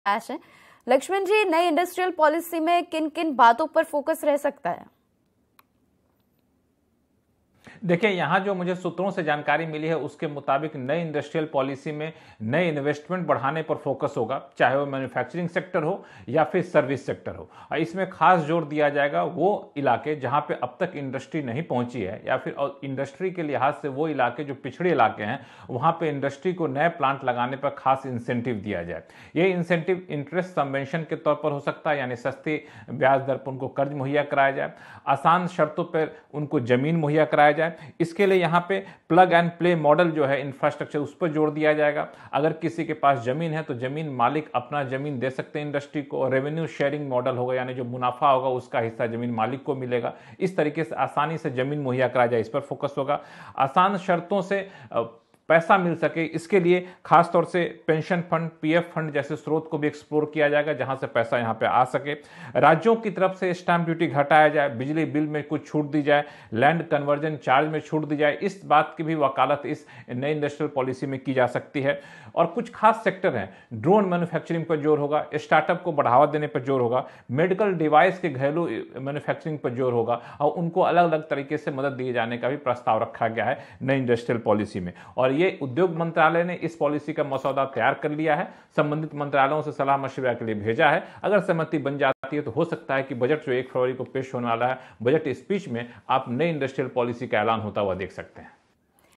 CNBC BAJAR | BAJAR EXCLUSIVE | BUDGET EXCLUSIVE | BUDGET 2023 | NEW INDUSTRIAL POLICY
CNBCBajar | બજેટમાં સરકાર નવી ઇન્ડસ્ટ્રીયલ પૉલિસીની જાહેરાત કરી શકે છે, CNBC-બજારના આ એક્સક્લુઝિવ સમાચાર પર વધુ જાણકારી આપી રહ્યાં છે ઇકોનોમિક પૉલિસી એડિટર લક્ષ્મણ રોય